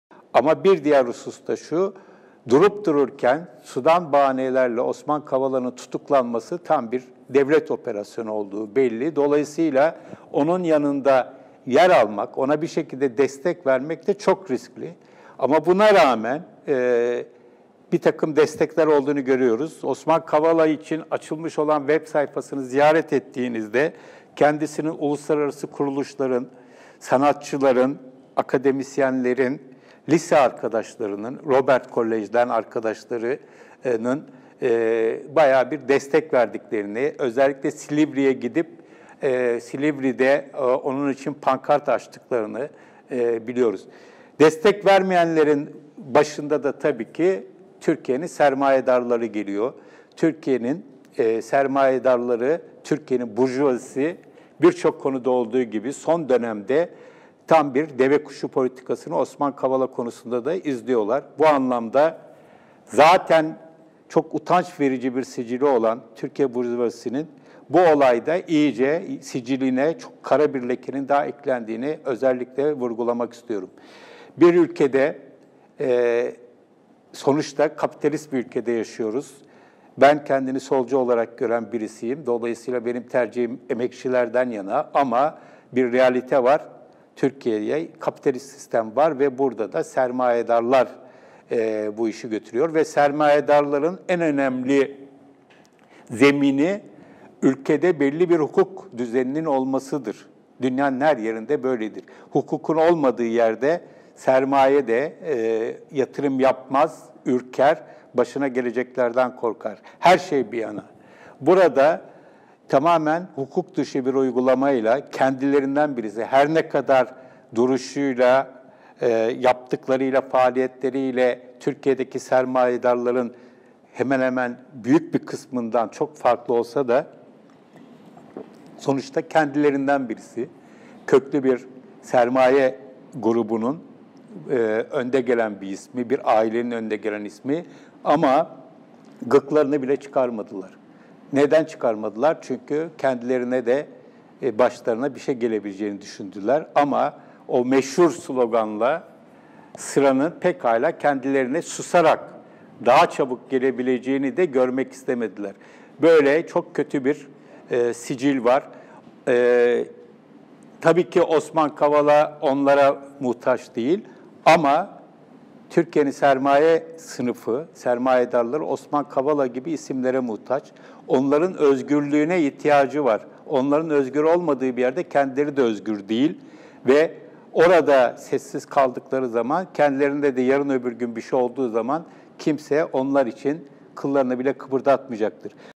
"Burjuvazi, birçok konuda olduğu gibi Kavala olayında da devekuşu politikası izledi”
Ruşen Çakır: “Türkiye’nin burjuvazisi, birçok konuda olduğu gibi Osman Kavala olayında da devekuşu politikası izledi”